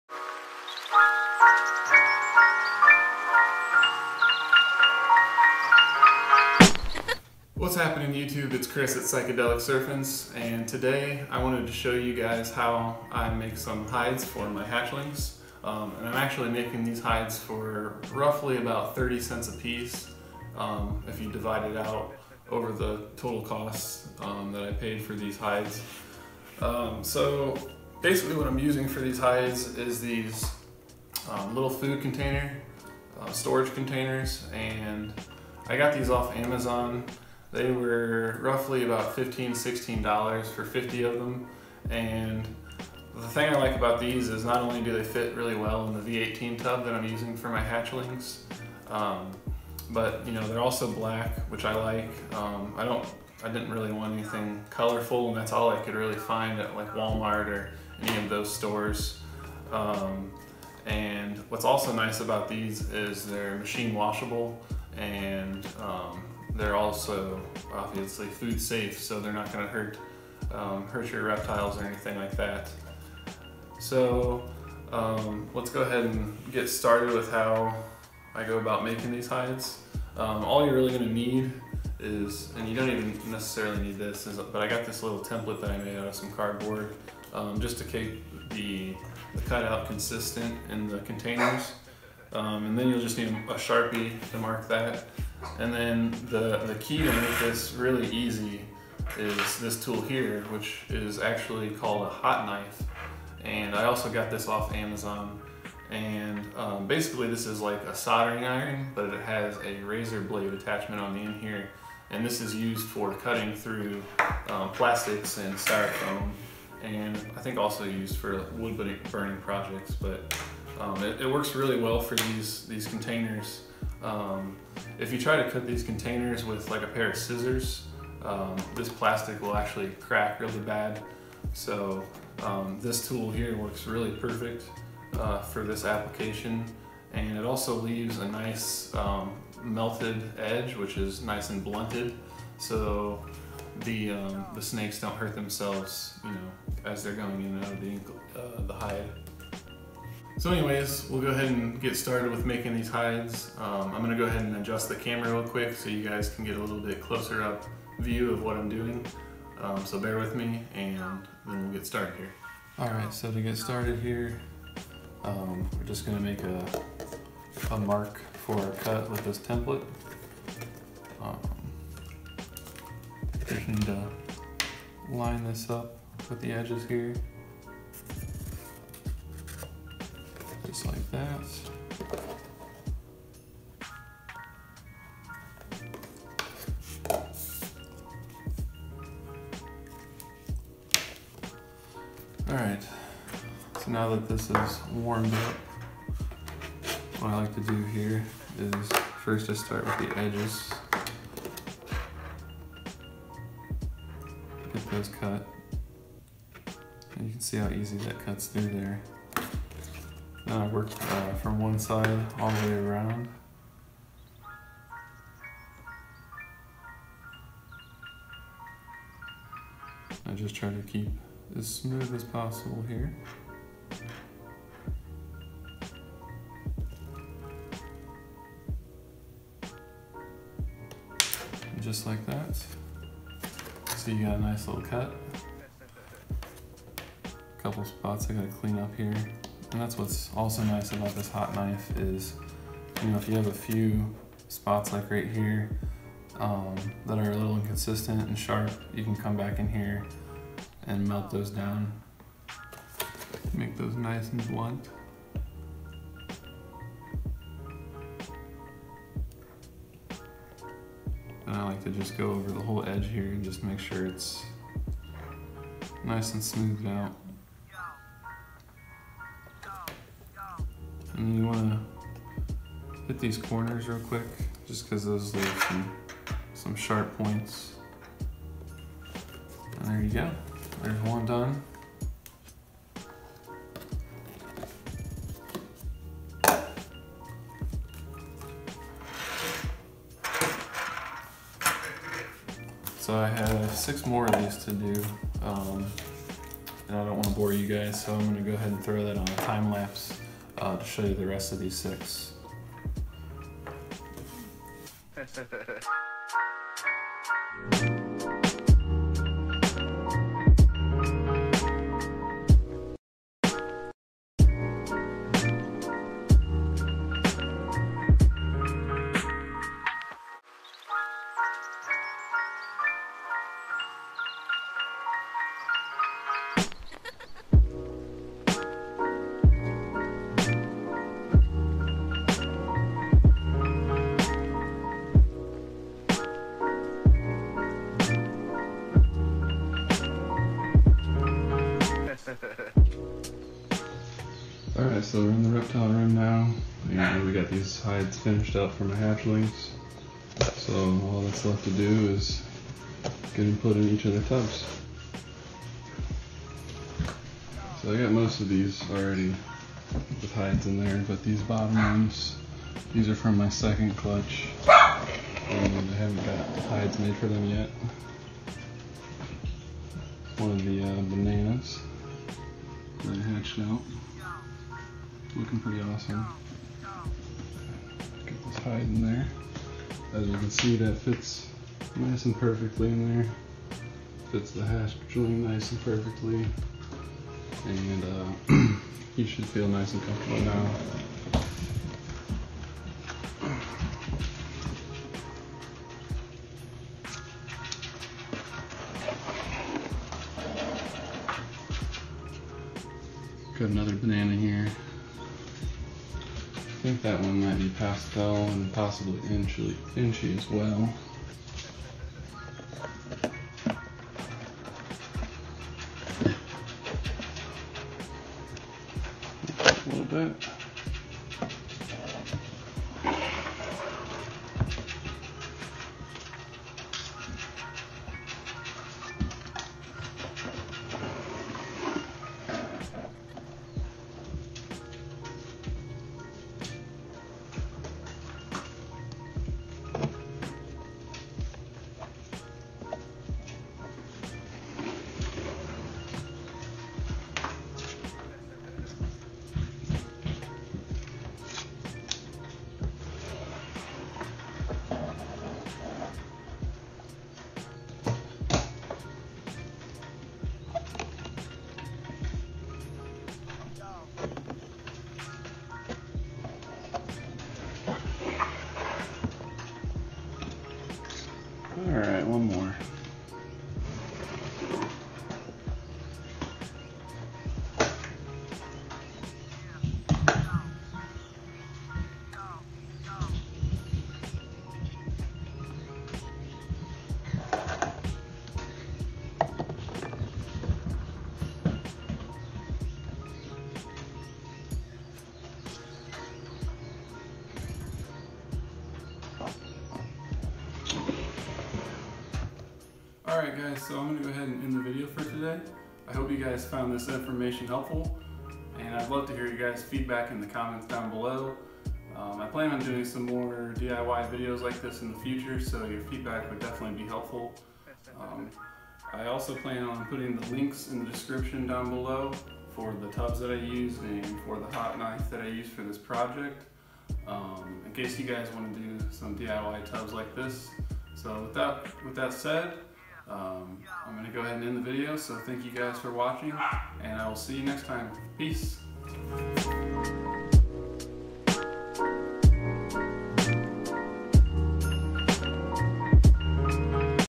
DIY Snake Hide - How to Make for $0.30
What’s happening YouTube! Today I am showing you how to make your own DIY snake hides for around $0.30 a piece. In this video I show you how I make hides for my hatchlings but this method can be applied to any size container if you need to make larger hides. I hope you find this information valuable!

The containers I used for this project:

The hot knife I used for this project: